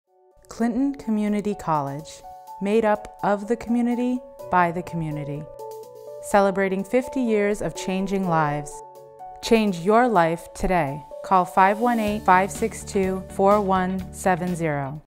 Clinton Community College - 50 Year Anniversary
Celebrating 50 years of changing lives.

Clinton Community College, a member unit of the State University of New York, is situated at Bluff Point, approximately 4 miles south of Plattsburgh, on forested heights overlooking Lake Champlain.

Clinton Community College (CCC) plays a unique role in the local community and within the state by offering traditional and nontraditional students access to a variety of educational programs taught by a highly dedicated faculty. Many CCC students attend for two years, earn a degree or certificate, then transfer to a four-year university. Other students graduate and enter directly into the workforce or attend CCC for a short period of time to meet a specific educational need. In the fall of 2015, Clinton Community College enrolled approximately 2,400 full-time and part-time students.